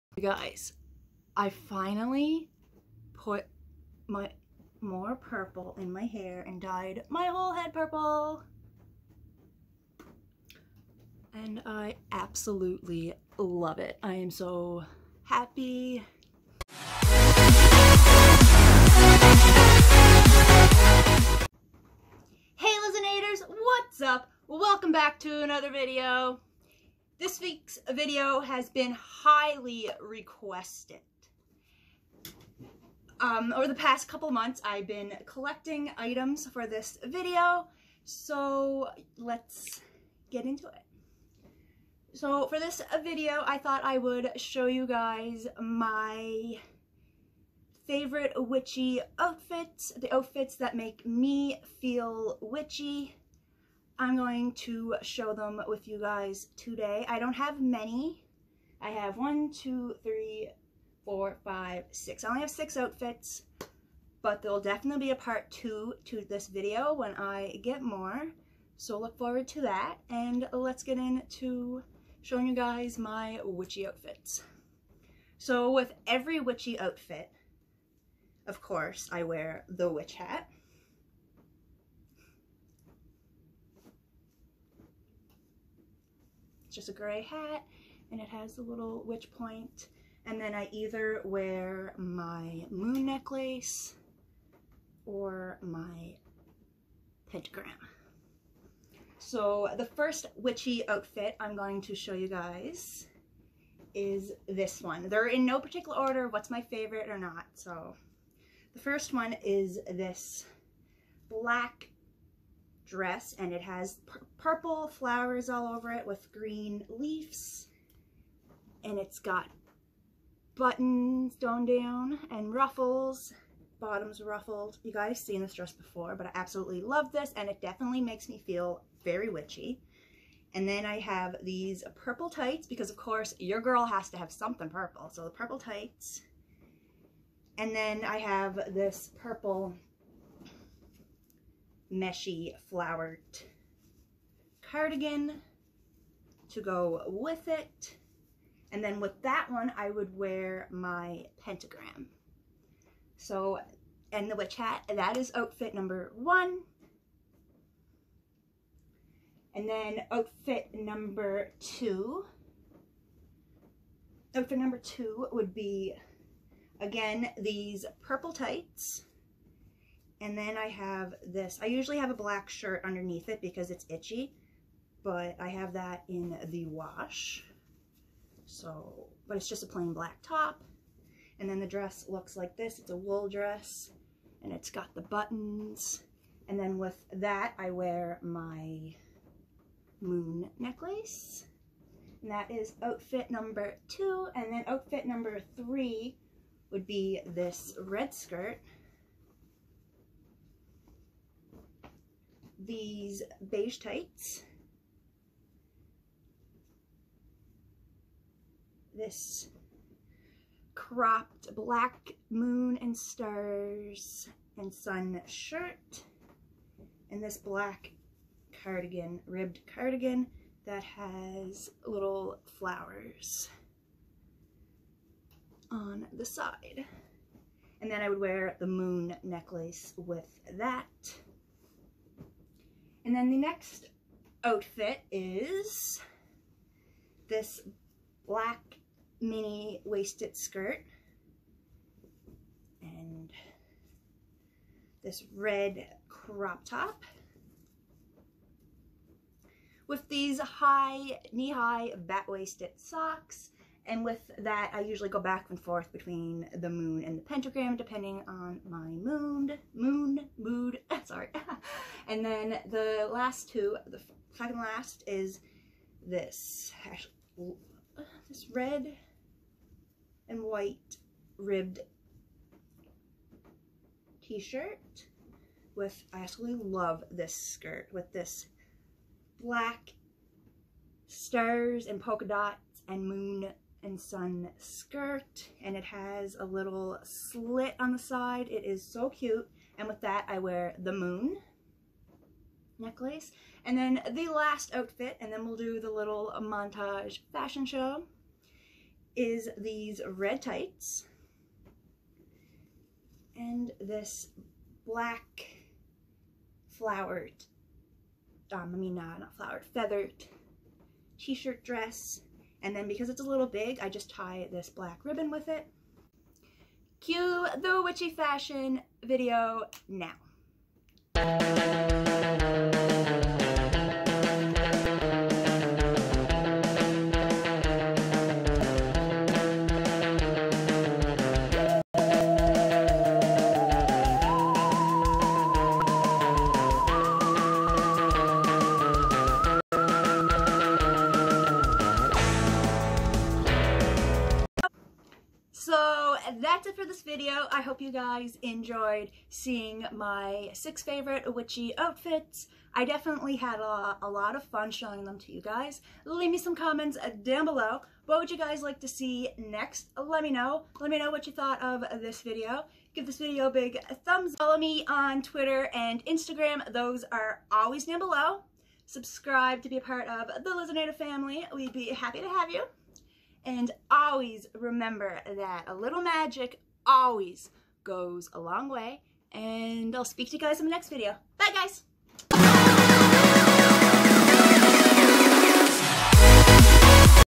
My Favorite Witchy Outfits
Hello and welcome to the Lizzanator Family if you're new! xo

In this video.. I finally show you my favorite witchy outfits! I had a blast.
Enjoy! xo

Leave me some comments below
Give this video a big thumbs up

peace out
xoxo
LizzAnn